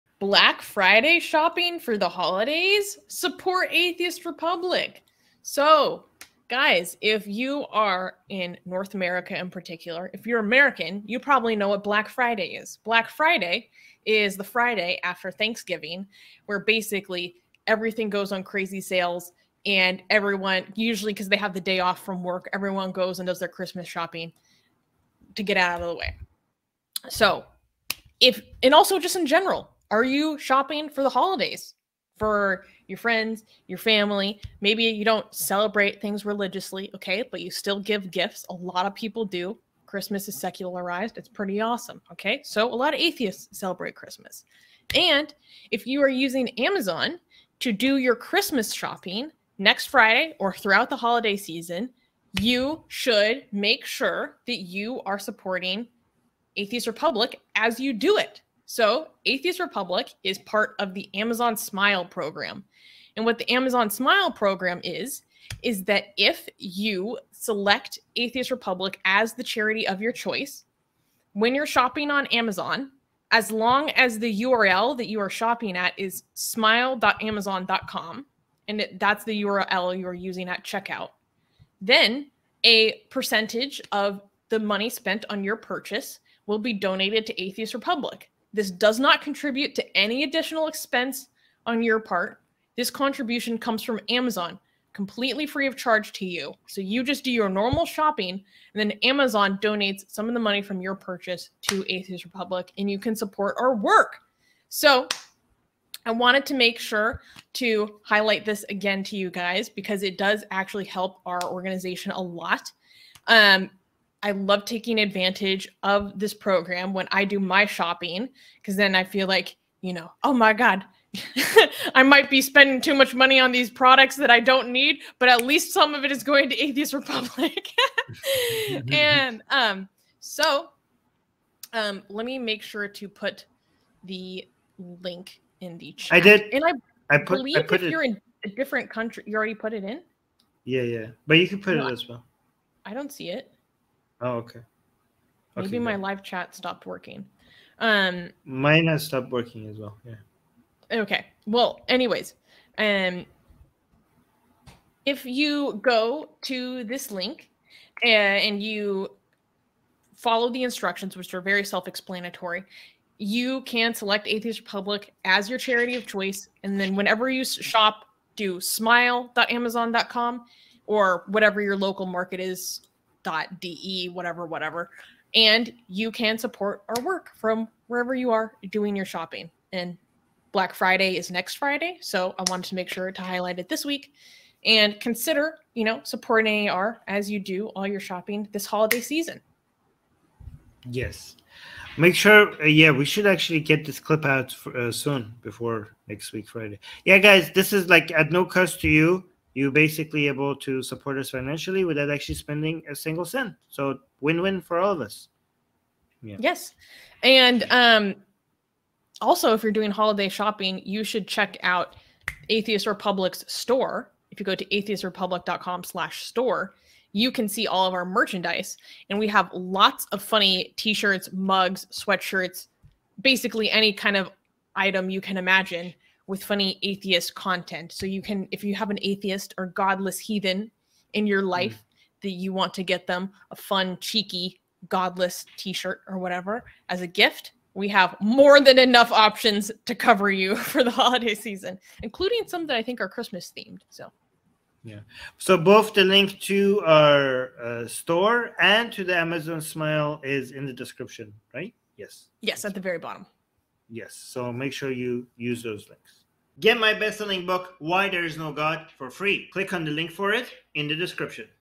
Black Friday Shopping For The Holidays? Support Atheist Republic!
Are you going to take advantage of Black Friday deals or do some gift shopping for the holiday season? Did you know that you can support Atheist Republic while you do your holiday online shopping on Amazon? We are part of Amazon’s Smile program, which means that when you select Atheist Republic as the Smile Charity of your choice, Amazon donates a percentage of your purchase to Atheist Republic at no additional cost to you! To automatically select Atheist Republic as the charity you support on future purchases, simply follow this Then, every time you shop on Amazon, make sure you replace the “ in the URL with “smile.” and you have activated the donation process! To make it easier, you can bookmark “smile.Amazon.com” so you don’t have to remember to change the URL. This also works outside the United States market, like Amazon.de, Amazon.in, etc. Please support Atheist Republic’s work of building an international godless community by selecting us as your Amazon Smile Charity of choice!
Looking for a gift for the godless heathen in your life? Make sure to check out Atheist Republic’s merchadise store full of witty and cool atheist shirts, sweatshirts, mugs, and lots of other items!